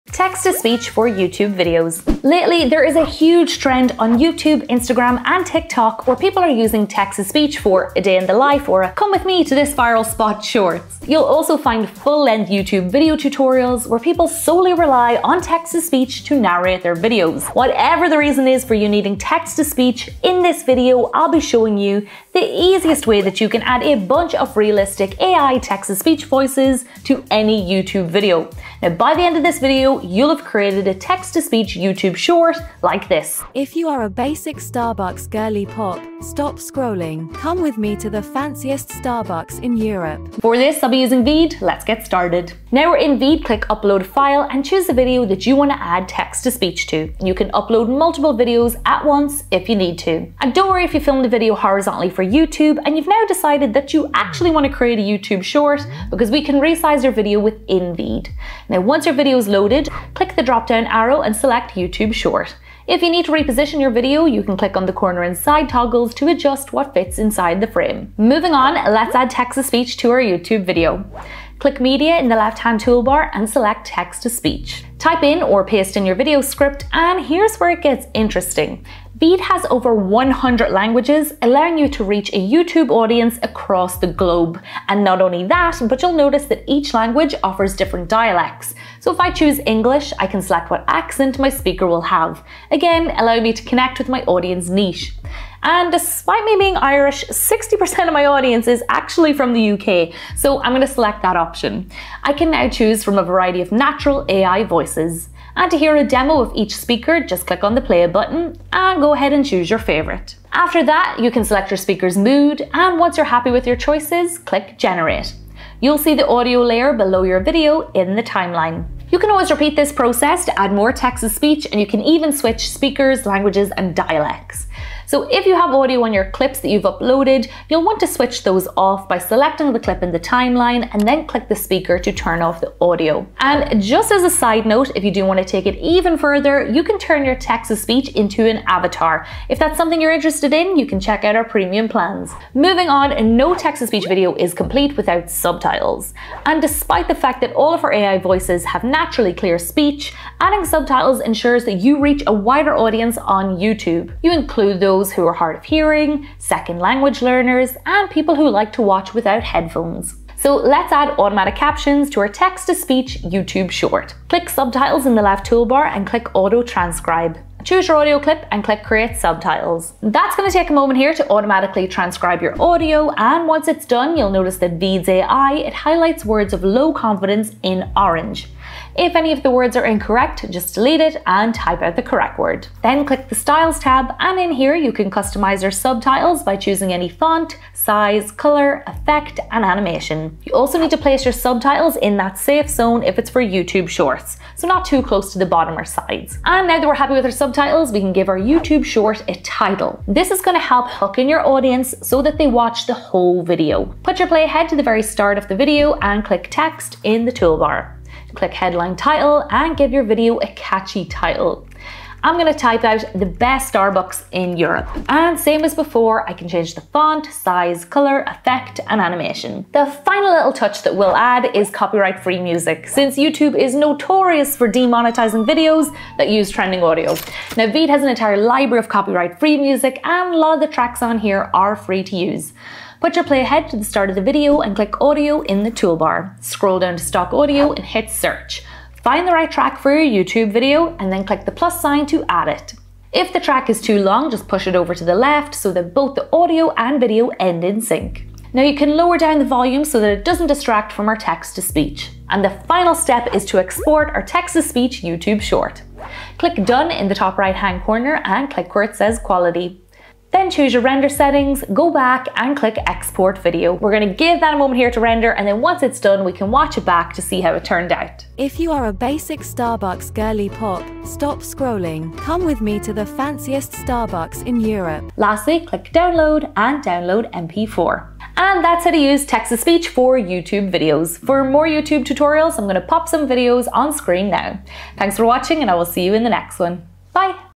Text to Speech for YouTube Videos | FAST & EASY
In this video I'll show the fastest way to create a text to speech video for YouTube. Lately, there’s a HUGE trend on YouTube, Instagram & TikTok where people are using text to speech for ‘a day in the life’ or a ‘come with me to this viral spot’ shorts. You’ll also find full-length YouTube videos of tutorials where people solely rely on text-to-speech to narrate their videos.  Whatever your reason is for needing text-to-speech, in this video I’ll show you, the EASIEST way that you can add a bunch of realistic AI text-to-speech voices to ANY YouTube video. To get started just click the link above! 🔗⬆️

TEXT TO SPEECH FOR YOUTUBE VIDEOS
0:00 Text to Speech for YouTube Videos Outcome
0:42 Import your YouTube video for text to speech
1:08 How to add text to speech to YouTube Videos
2:22 How to add subtitles to text to speech for YouTube
3:20 How to give your text to speech YouTube video a title.
3:39 Adding Music to Your Text to Speech YouTube Video
4:12 How to render & export your YouTube video
4:38 Outro

If you have any questions about how to add text to speech for YouTube videos, please pop them in the comments section! 💖

Lauren  & VEED.IO